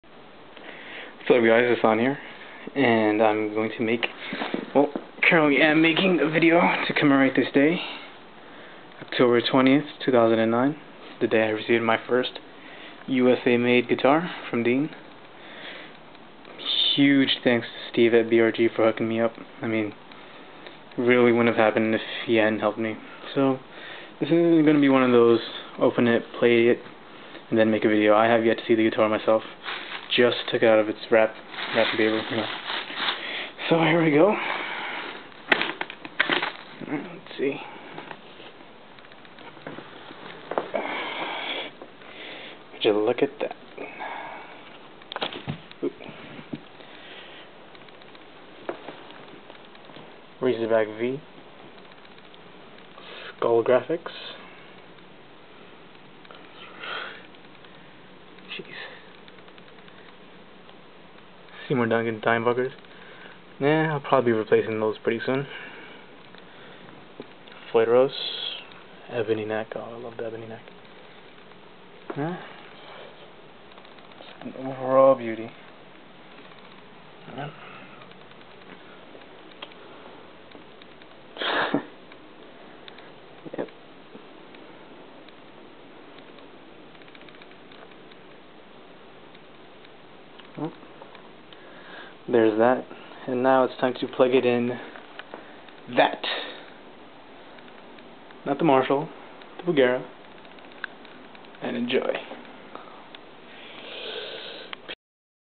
New Guitar Day! Dean USA Razorback V
No cover this time.

Just wanted to keep a video log of this day.  10/20/09 - the day I got my first USA Dean.

And I really didn't open it before filming.  I first time I saw it was while I was filming this

Thank you to all my subscribers!  Your help is much appreciated.